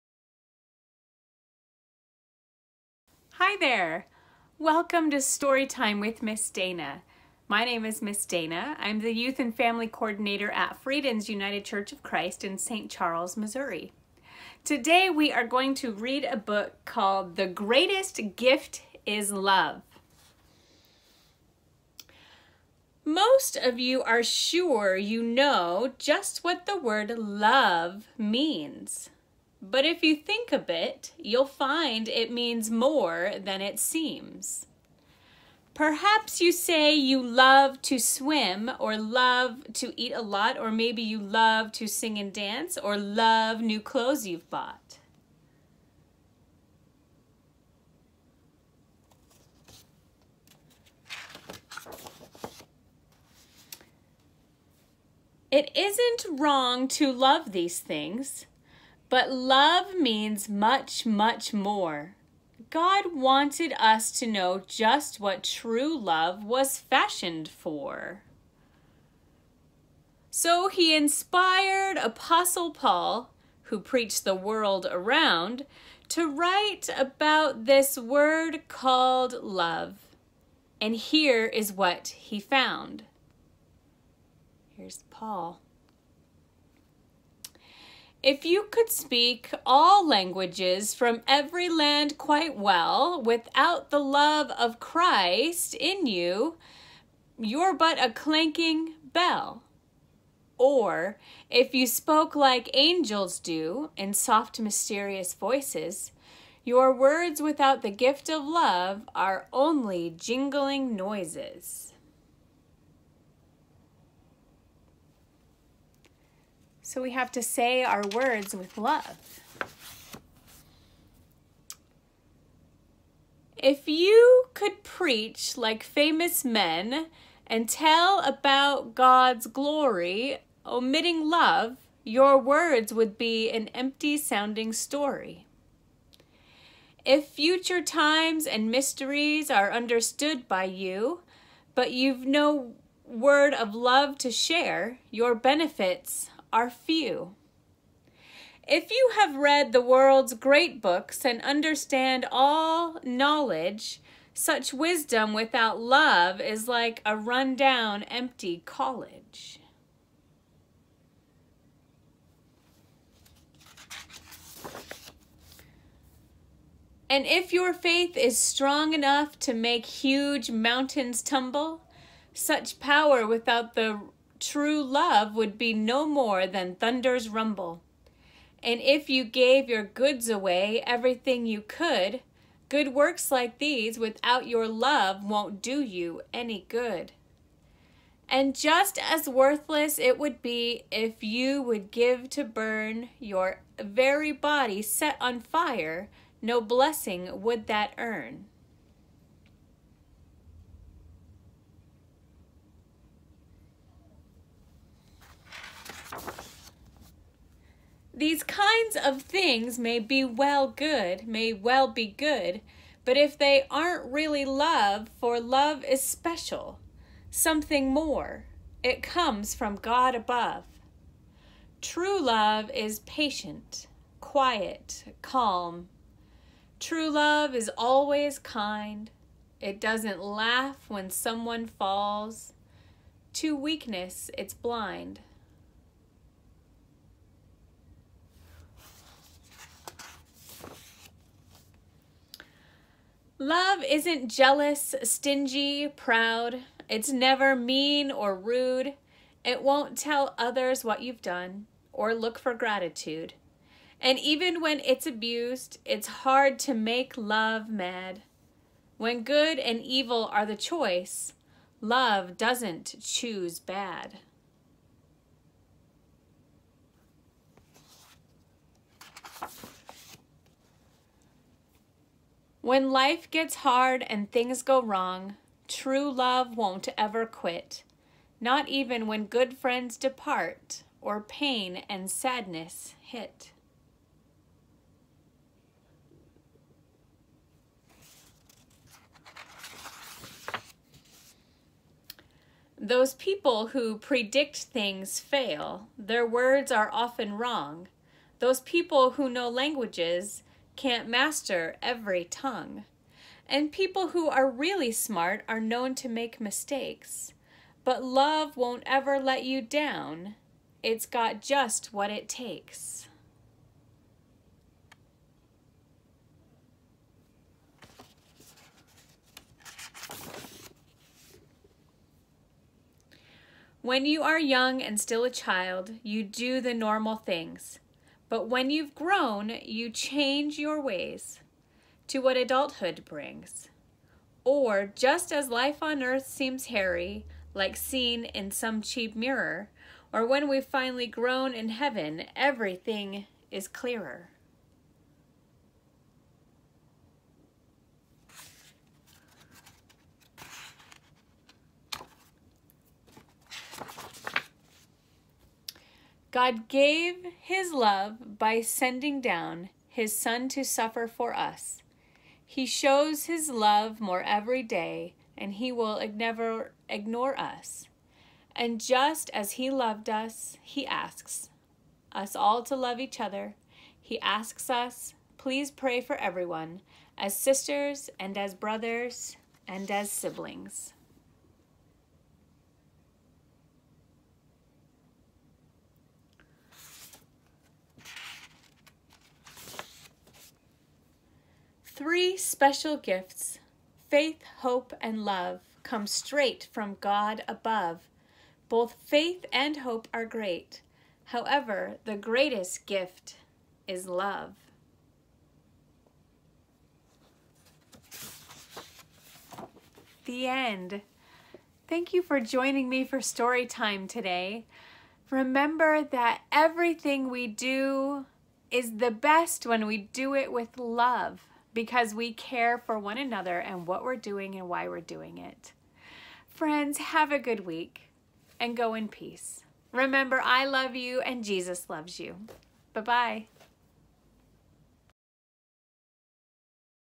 Friedens Story Time - The Greatest Gift is Love
The Greatest Gift is Love
1 Corinthians 13 for children
Written by Robert Baden
Illustrated by Gordon Williams
ARCH Books
1985. Concordia Publishing House, St. Louis